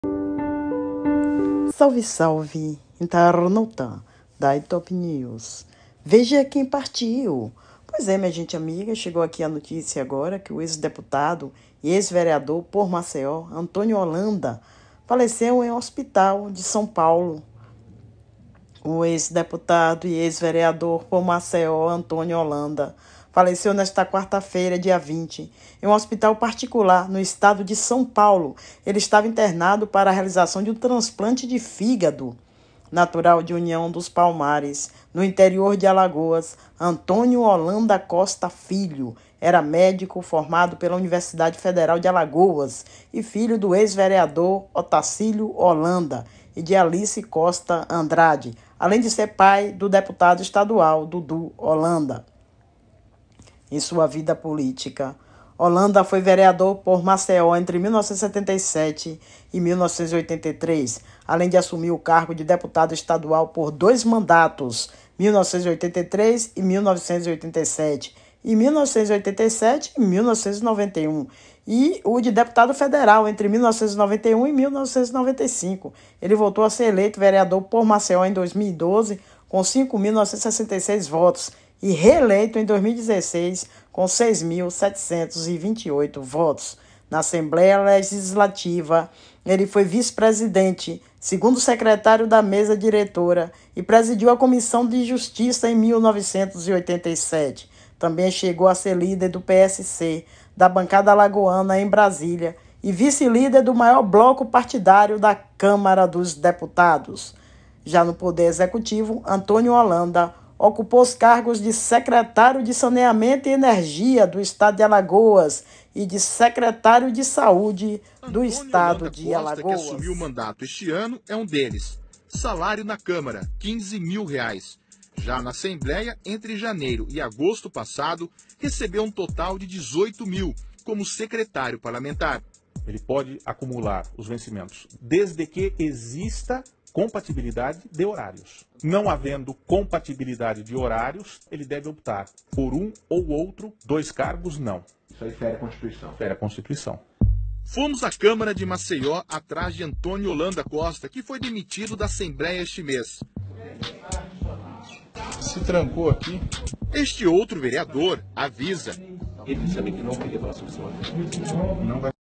Faleceu o ex-deputado e ex-vereador por Maceió, Antônio Hollanda
O ex-deputado e ex-vereador por Maceió Antônio Hollanda morreu aos 74 anos, nesta quarta-feira (20), no hospital Sírio Libanês, em São Paulo. Ele estava internado para a realização de um transplante de fígado.
Natural de União dos Palmares, no interior de Alagoas, Antônio Hollanda Costa Filho era médico formado pela Universidade Federal de Alagoas (UFAL) e filho do ex-vereador Otacílio Holanda e de Alice Costa Andrade, além de ser pai do deputado estadual Dudu Hollanda.
Em sua vida na política, Hollanda foi vereador por Maceió entre 1977 e 1983, além de assumir o cargo de deputado estadual por dois mandatos (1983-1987 e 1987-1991) e o de deputado federal entre 1991 e 1995. Ele voltou a ser eleito vereador por Maceió em 2012, com 5.966 votos, e reeleito em 2016, com 6.728 votos.